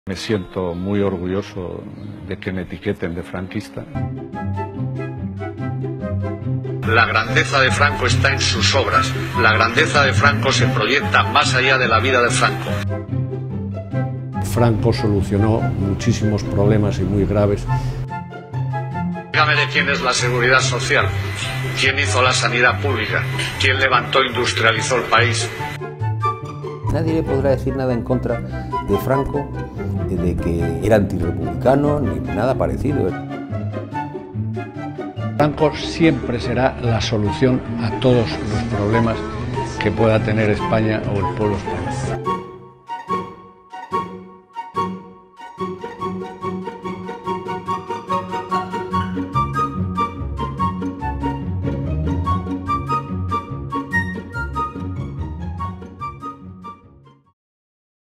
Las frases más vergonzosas de los representantes de la Fundación Francisco Franco
Los representantes de la Fundación Francisco Franco se pasean por televisiones y radios defendiendo y enalteciendo la figura del dictador. En Alemania algo similar, aplicado al régimen nazi, estaría penado con hasta tres años de prisión.